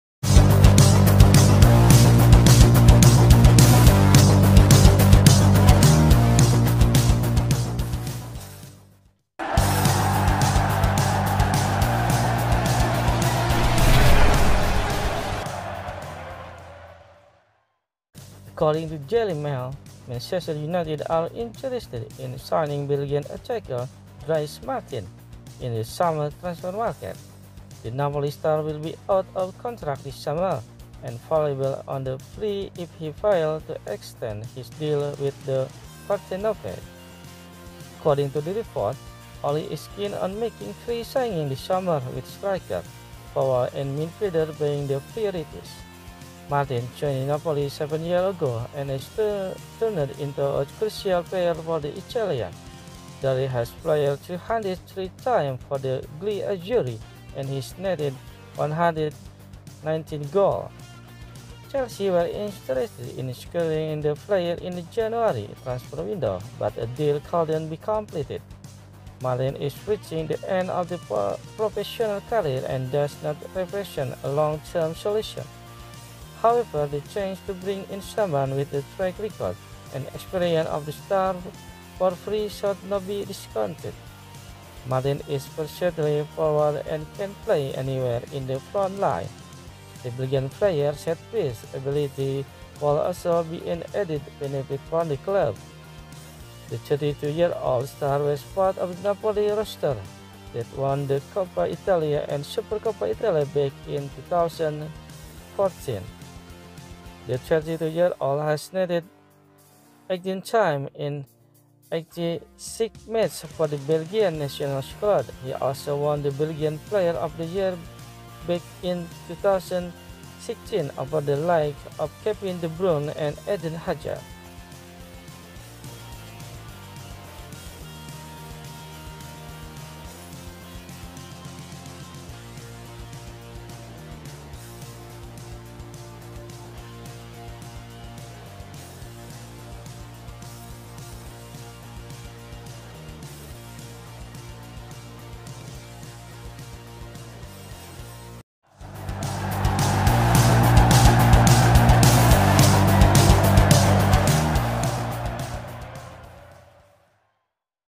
Manchester United agreed in signing soon-to-be free agent Belgian Attacker
According to Daily Mail, Manchester United are interested in signing Belgian attacker Dries Mertens in the summer transfer market.

The Napoli star will be out of contract this summer and available on a free if he fails to extend his deal with the Partenopei.